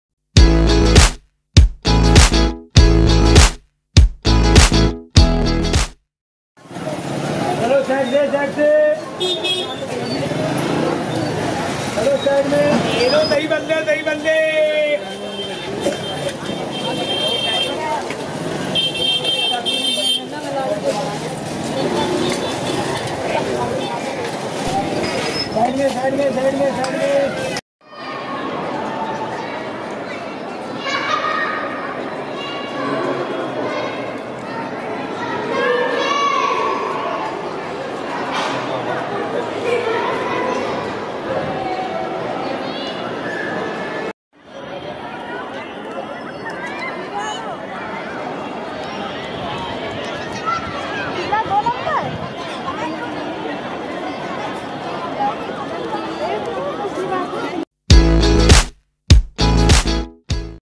Streets of Old Delhi | Jama Masjid | Purani Dilli Ki Ek Jhalak
This video shows a ride through Old delhi popularly known around the world as Purani Dilli. This part of Delhi city is famous for it's rich culture and tradition. It's heritage and legacy is historically significant. Old Delhi is famous for it's ethnic culture where almost people for all faiths are found. Muslim population is found mostly nearby Jama Masjid and Daryaganj areas and is famous for it's variety of Non vegetarian food and sweet delicacies. There are a lot of markets and places to visit nearby, most popular being Jama Masjid. You can visit Red Fort, Daryaganj market, Nai Sarak (Book shops and wedding material purchase like suit, lehenga, chunni, bride and groom dresses), Chandni Chowk (Markets), Ballimaran (Mirza Ghalib ki Haveli).